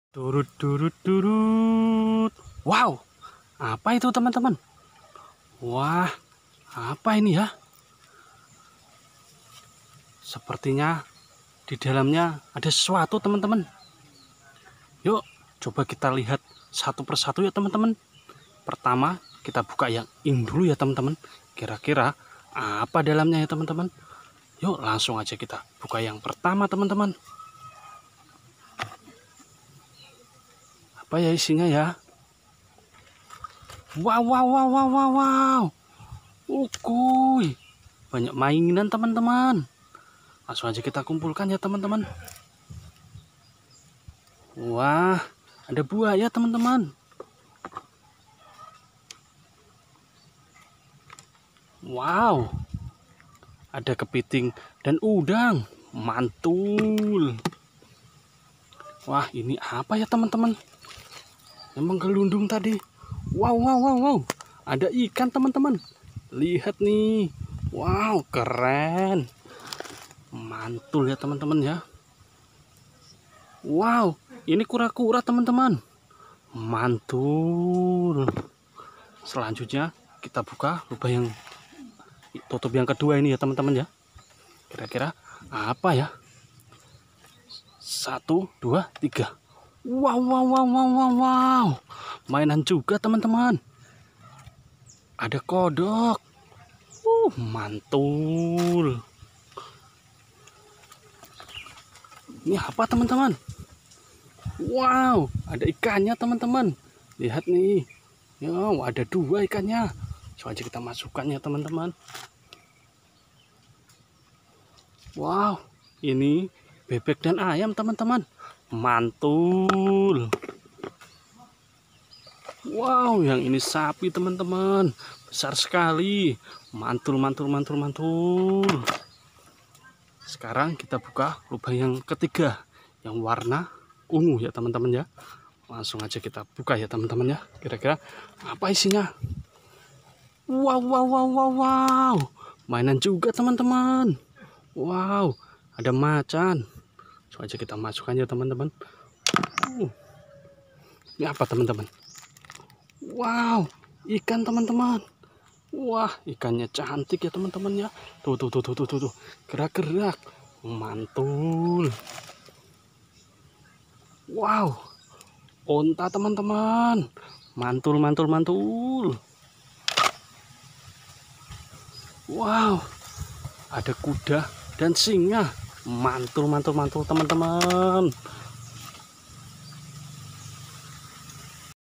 Menemukan mainan gajah buaya onta dan ikan hias( part 14)
Mencari dan menemukan banyak mainan hewan dan ada ikan jiwanya juga...